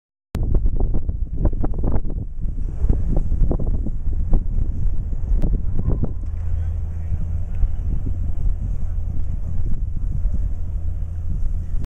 Tosa, La Masella
One of the highlights of skiing is you get to enjoy fresh air and panoramic views like this, 2,535m ASL.  This was captured at the peak of La Masella, a ski resort located near Barcelona (only an hour and a half drive away).